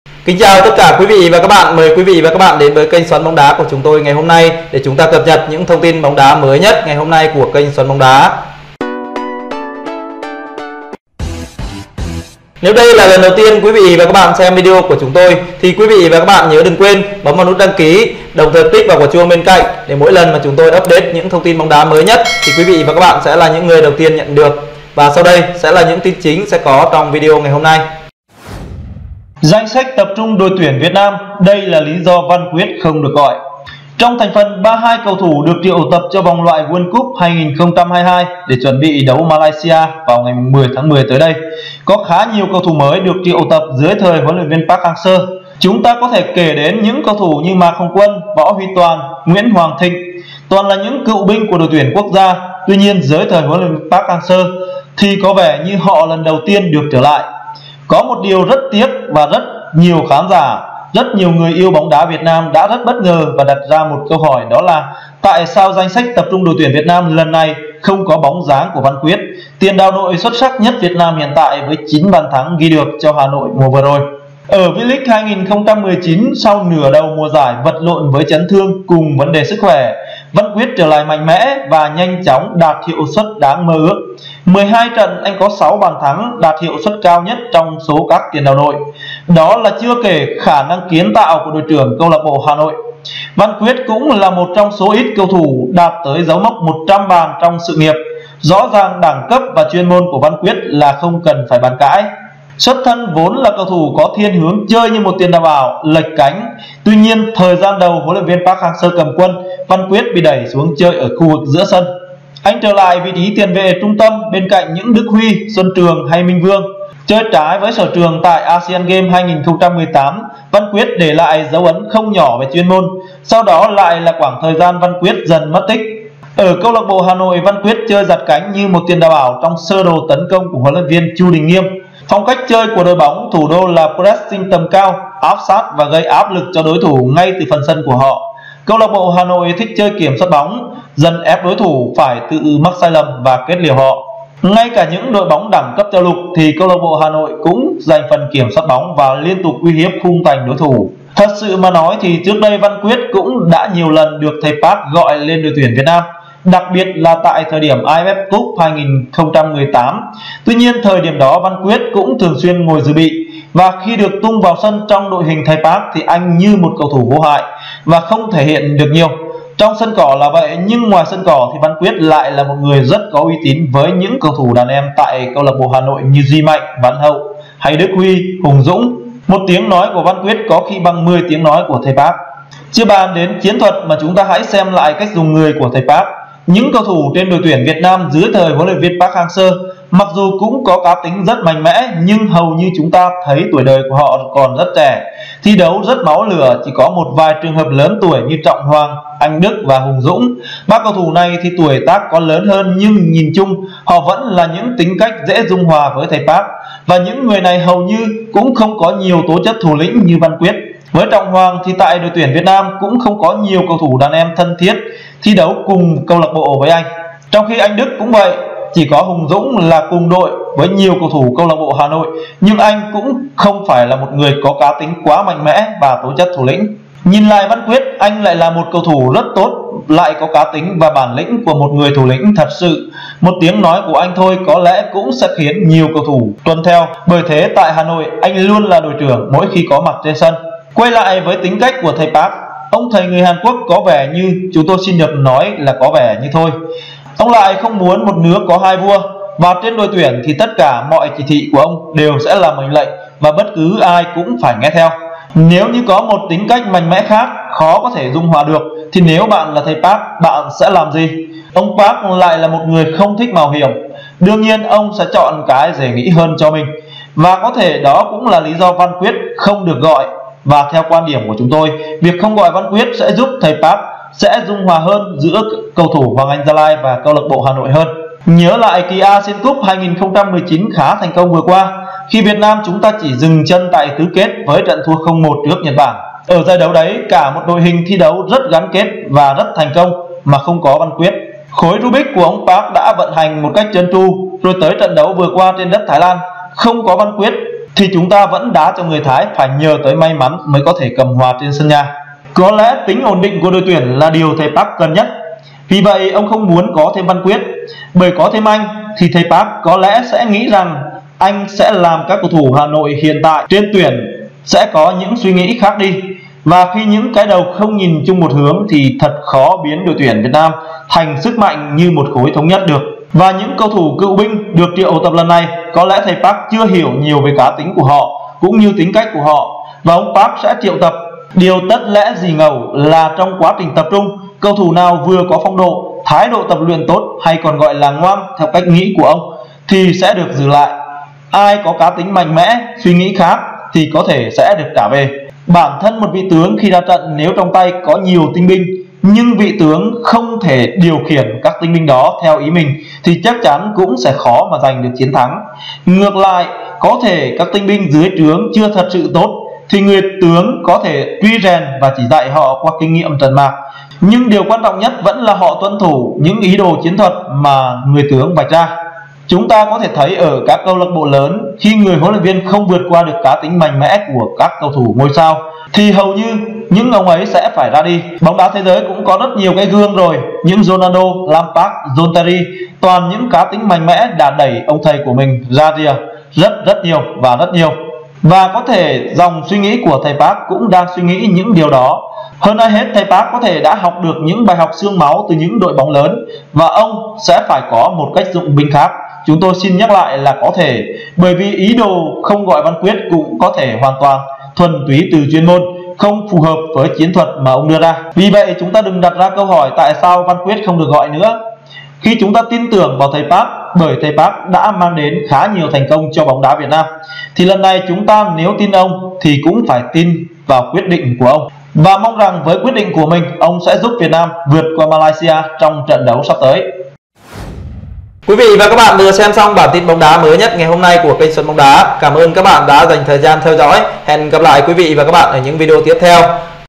Danh Sách Đội Tuyển Việt Nam Đây Là Lý Do Văn Quyết Không Được gọi
Danh Sách Đội Tuyển Việt Nam 32 cầu thủ được triệu tập cho vòng loại world cup 2022 để chuẩn bi đấu Malaysia vào 10/10 Tới có khá nhiều cầu thủ mới được triệu tập dưới thời hlv park hang seo. Chúng ta có thể kể đến Những cầu thủ như Mạc Hồng Quân, Võ Huy Toàn, Nguyễn Hoàng Thịnh Toàn là những cựu binh của đội tuyển quốc gia tuy nhiên dưới thời HLV Park Hang Seo Thì có vẻ như họ lần đầu tiên được trở lại Danh Sách Đội Tuyển Việt Nam. Có 1 điều rất tiếc và rất nhiều khán giả, nhiều người yêu bóng đá việt nam đã rất bất ngờ và đặt ra 1 câu hỏi đó là Tại sao Danh Sách Đội Tuyển Việt Nam lần này không có bóng dáng của Văn Quyết. Tiền đạo nội xuất sắc nhất việt nam hiện tại với 9 bàn thắng ghi được cho Hà Nội mùa vừa rồi.
Thật sự mà nói thì trước đây Văn Quyết cũng đã nhiều lần được thầy park gọi điền tên vào Danh Sách Đội Tuyển Việt Nam. Đặc biệt là tại thời điểm AFF cup 2018. Tuy nhiên thời điểm đó Văn Quyết cũng thường xuyên dự bị, và khi được tung vào sân trong đội hình thầy park thì anh như 1 cầu thủ vô hại và không thể hiện được nhiều. Trong sân cỏ là vậy nhưng ngoài sân cỏ thì Văn Quyết lại là 1 người rất có uy tín với những cầu thủ đàn em tại câu lạc bộ hà nội như Duy Mạnh, Văn Hậu, Đức Huy, Hùng Dũng... Một tiếng nói của Văn Quyết có khi bằng 10 tiếng nói của thầy park.
Quay lại với tính cách của thầy park Ông Thầy người hàn quốc có vẻ như (chúng tôi xin được nói là có vẻ như thôi) ông lại không muốn 1 nước có 2 vua. Và trên đội tuyển thì tất cả mọi chỉ thị của ông đều sẽ là mệnh lệnh và bất cứ ai cũng phải nghe theo. nếu như có thêm 1 tính cách mạnh mẽ khác khó có thể dung hòa được thì nếu bạn là thầy park bạn sẽ làm gì?? Ông park lại là người ko thích mạo hiểm nữa. Đương nhiên ông sẽ chọn cái dễ nghĩ hơn cho mình và có thể đó cũng là lý do Văn Quyết không được gọi và theo quan điểm của chúng tôi việc không gọi văn quyết sẽ giúp thầy park dễ dung hòa hơn giữa các cầu thủ HAGL và CLBHN hơn. Nhớ lại kỳ asian cup 2019 khá thành công vừa qua khi việt nam chúng ta chỉ dừng chân tại tứ kết với trận thua 0-1 trước nhật bản Ở giải đấu đấy cả 1 đội hình thi đấu rất gắn kết và rất thành công khi mà Danh Sách Đội Tuyển Việt Nam ko có văn quyết.
Khối rubic của ông park đã vận hành 1 cách trơn tru. rồi tới trận đấu vừa qua trên đất thái lan ko có văn quyết thì chúng ta vẫn đá cho người thái phải nhờ tới may mắn mới có thể cầm hòa trên sân nhà.
Về những cầu thủ cựu binh được triệu tập Danh Sách Đội Tuyển Việt Nam có lẽ thầy park chưa hiểu nhiều về cá tính cũng như tính cách của họ và ông park sẽ triệu tập, điều tất lẽ dĩ ngẫu là trong quá trình tập trung cầu thủ nào vừa có phong độ, thái độ tập luyện tốt hay còn gọi là "ngoan" theo cách nghĩ của ông thì sẽ được giữ lại, ai có cá tính mạnh mẽ suy nghĩ khác thì có thể cũng sẽ được trả về.
Vì vậy chúng ta đừng đặt ra câu hỏi tại sao văn quyết không được gọi vào Danh Sách Đội Tuyển Việt Nam nữa, Khi chúng ta tin tưởng vào thầy park bởi thầy park đã mang đến khá nhiều thành công cho bóng đá việt nam, thì lần này chúng ta nếu tin ông thì cũng ta phải tin vào quyết định của ông.
Để xem đầy đủ quý vị và các bạn hãy xem video từ kênh xoắn bóng đá nhé.
Ngoài ra các bạn đừng quên đăng ký để nhận những tin bóng đá mới nhất